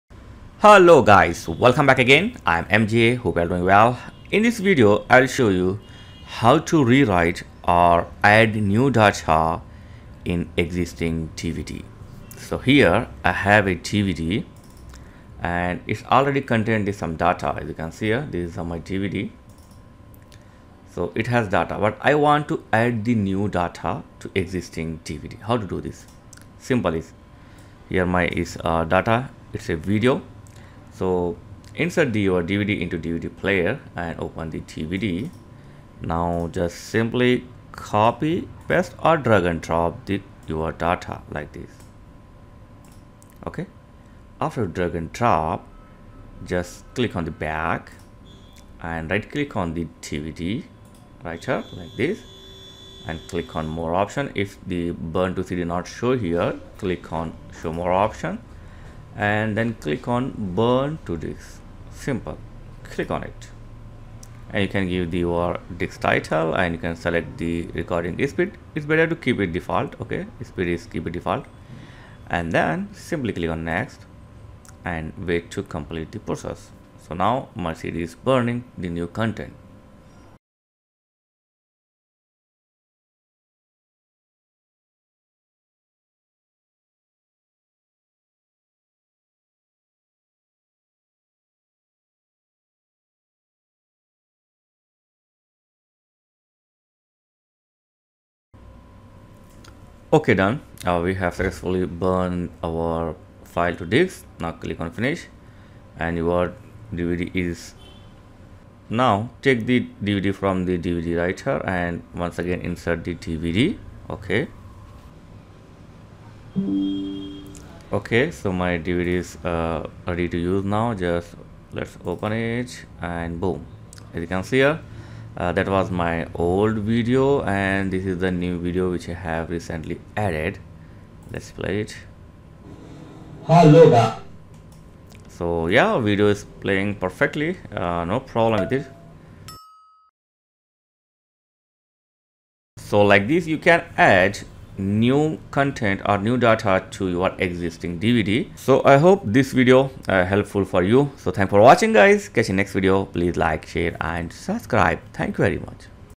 How to Add New Data Again & Again in Existing DVD Disc (Burn DVD Multiple Times)
Burn DVD Disc Again & Again (Add New Data in Old DVD Disc)